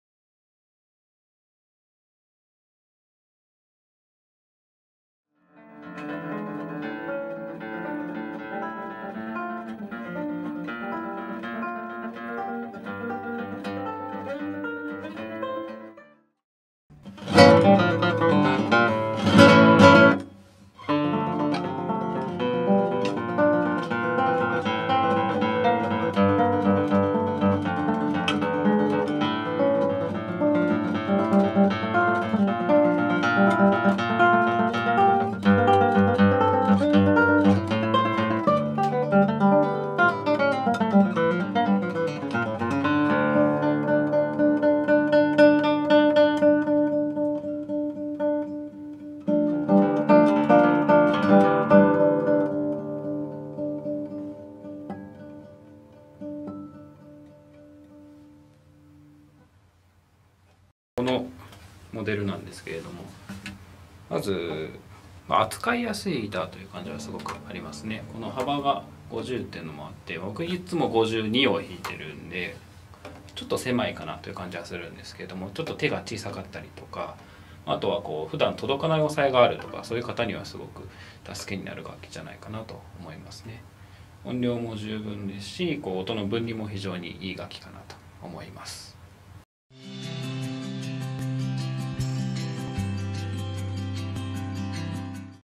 生田直基が解説する銘器の世界【 Juan Hernandez Maestro 松 French Polish 】
スペイン、ヴァレンシアの製作家「ホアン・エルナンデス」の最上位モデル「Maestro」。
その逸品をセラック塗装で仕上げた逸品です。

日本人の手にも馴染みやすく
普段エレキギターやアコースティックギターを使用している方にも使いやすいネックシェイプ。
比較的低価格ながらコンサートギターとしても使用できるクオリティを持つ上質なスパニッシュギターをお探しな方にも大変おススメな商品です。